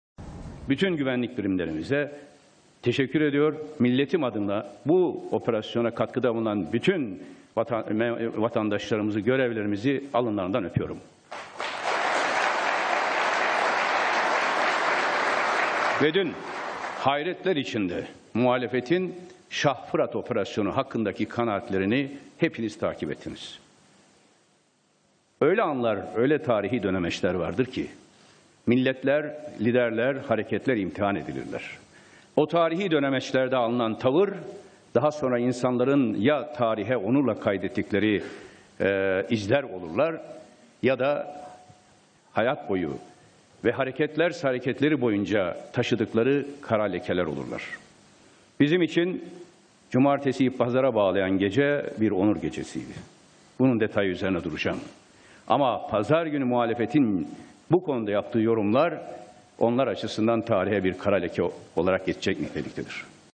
Davutoğlu Şah Fırat Operasyonunu Yapanları Alınlarından Öpüyorum,Şah Fırat,Süleyman Şah,shah
Davutoğlu- Şah Fırat Operasyonunu Yapanları Alınlarından Öpüyorum